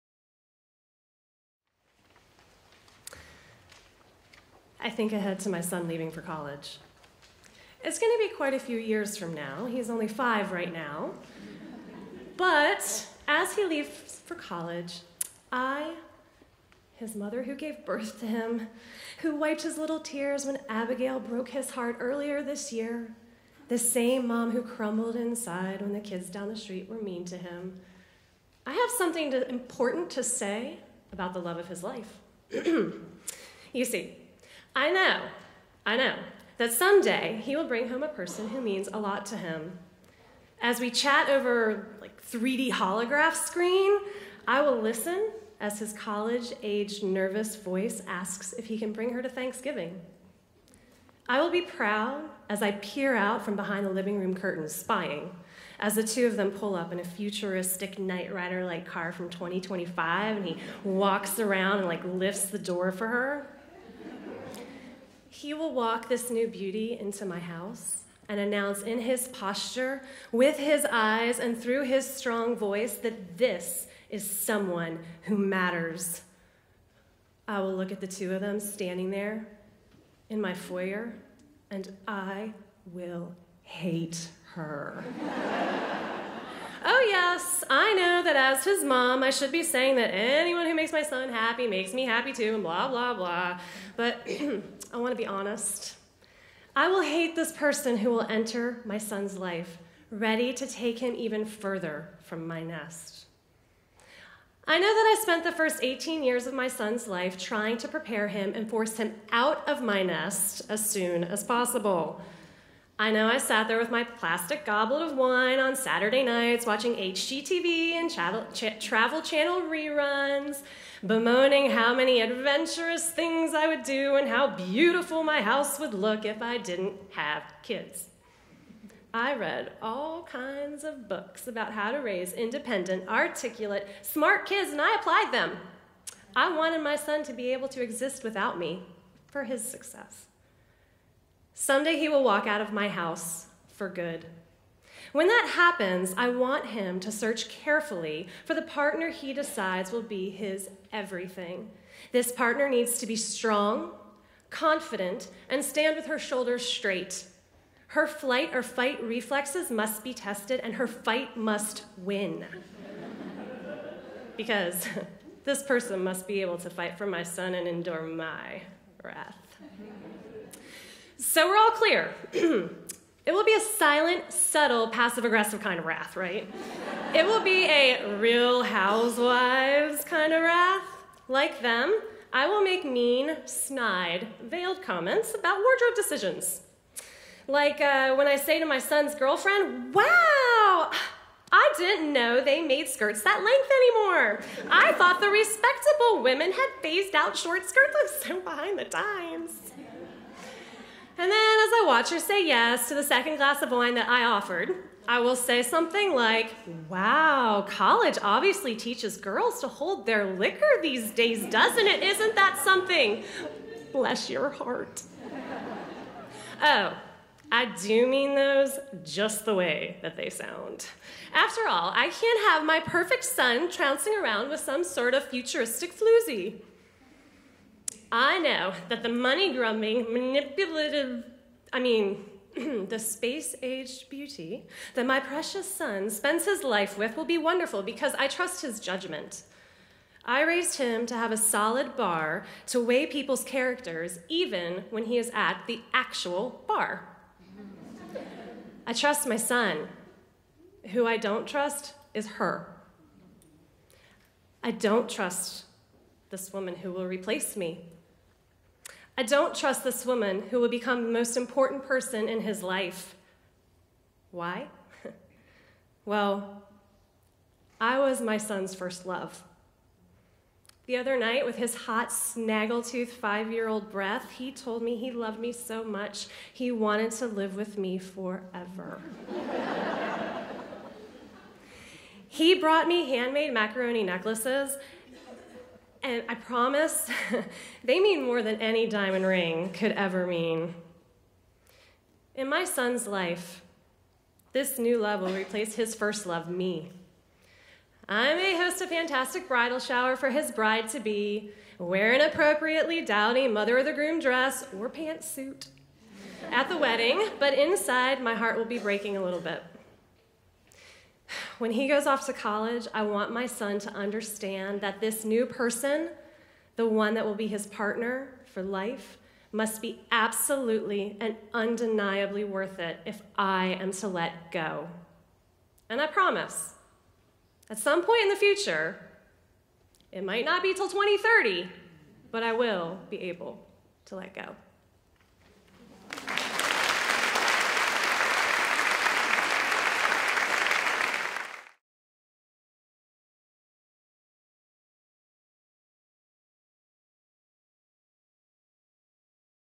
Allison Barrett Carter reading “When He Falls for Her”
A mother ponders how she will feel and behave when her son brings home the love of his life in the future.

LTYM: Raleigh-Durham ● 5/7 & 5/8/15 ● Kenan Hall, William Peace University